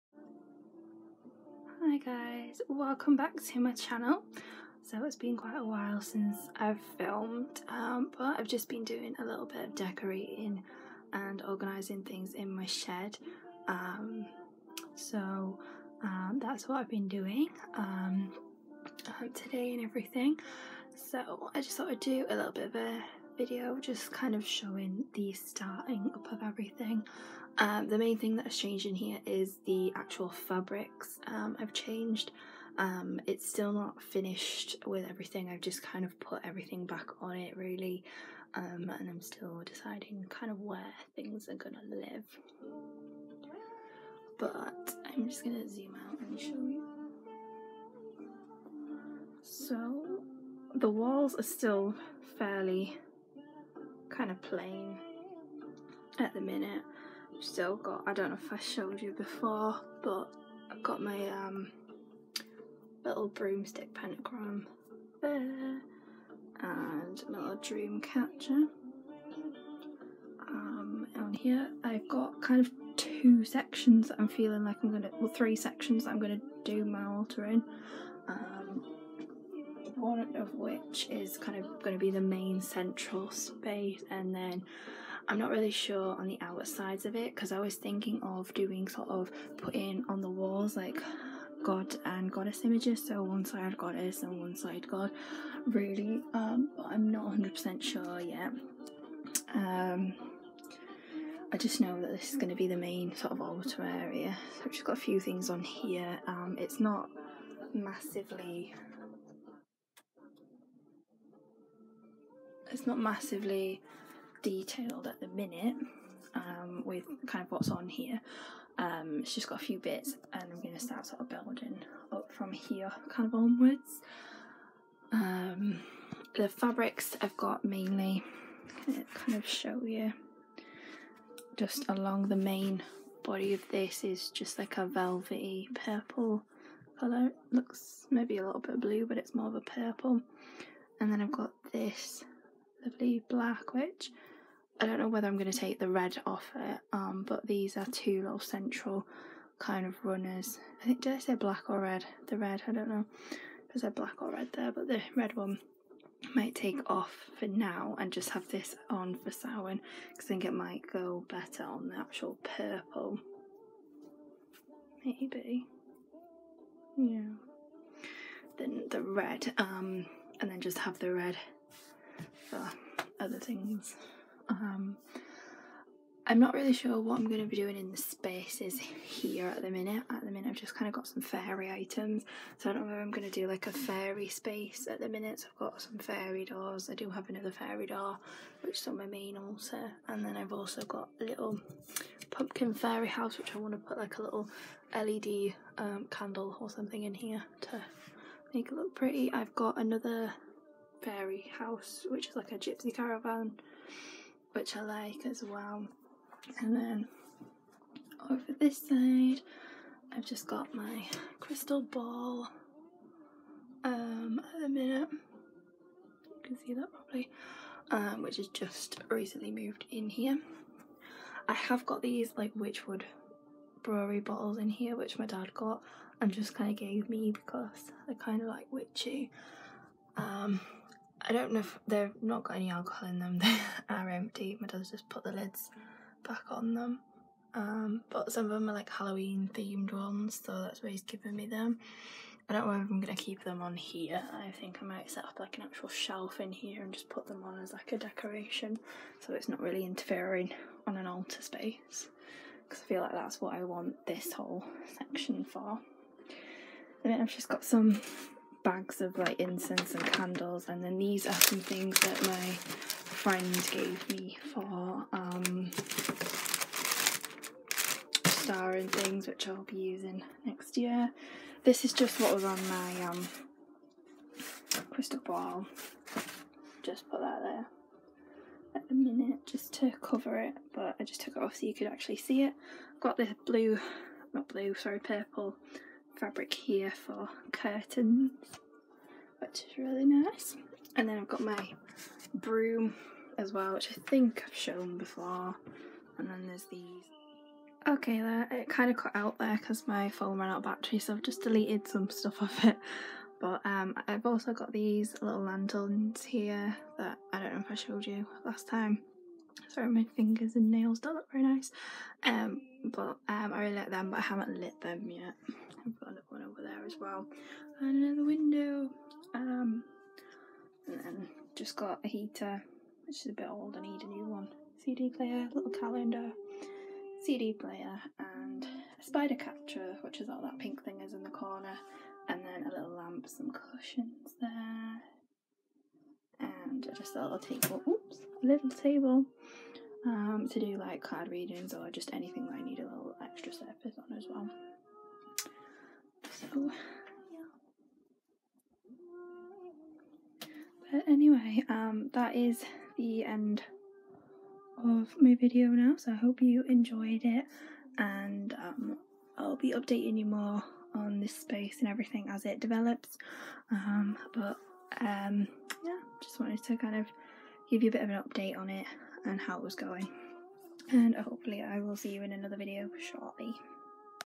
Sacred Space Tour & Update
Yes it’s been a while. Just been really busy and everything has been crazy and just not getting opportunities to film.

But what I have been up to is updating and organising my sacred space.

I do hope to get back into filming some more in the next couple of months and trying to have regular uploads.

I’m am fairly active on social media so feel free to get in touch.

Facebook: The Moonlight Faeire